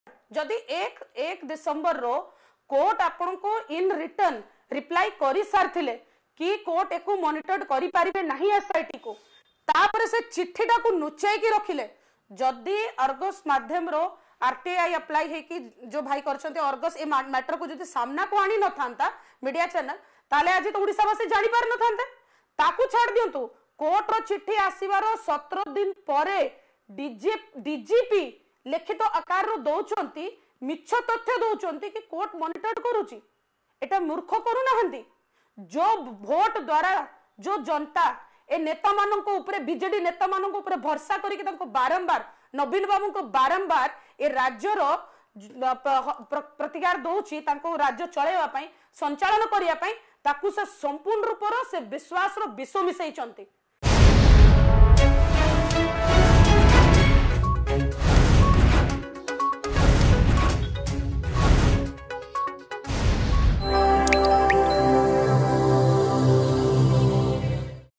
ରାଜ୍ୟବାସୀଙ୍କ ବିଶ୍ବାସରେ ବିଷ ମିଶାଉଛନ୍ତି ରାଜ୍ୟ ସରକାର
ରାଜ୍ୟବାସୀଙ୍କୁ ସନ୍ତୁଷ୍ଟ କରିପାରୁନାହାଁନ୍ତି ରାଜ୍ୟ ସରକାର ମତ ଦେଲେ ବିଜେପି ନେତ୍ରୀ ସିପ୍ରା ବାଜପେୟୀ.
ଅଧିକ ଖବର ପାଇଁ ଡ଼ାଉନଲୋଡ କରନ୍ତୁ “Argus News” ଆପ୍